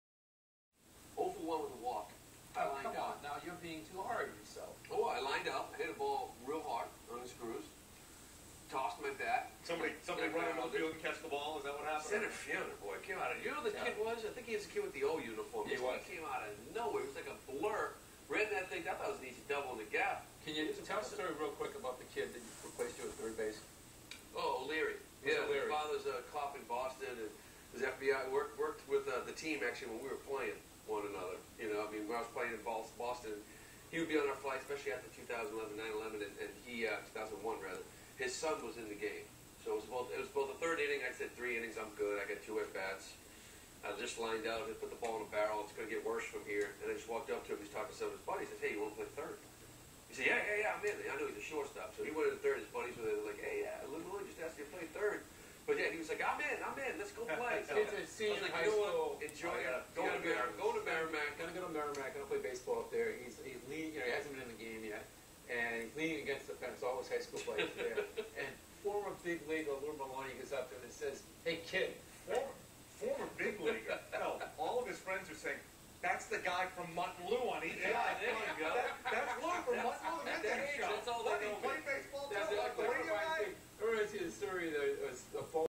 Lou Merloni References Kyle Schiavone's Web Gem
Former Red Sox utility infielder Lou Merloni references his only at-bat in the 2011 Old Time Baseball Game at St. Peter's Field when he was robbed of a extra bases when Newbury centerfielder Kyle Schiavone came out of nowhere to make a spectacular play on a line drive into left-center.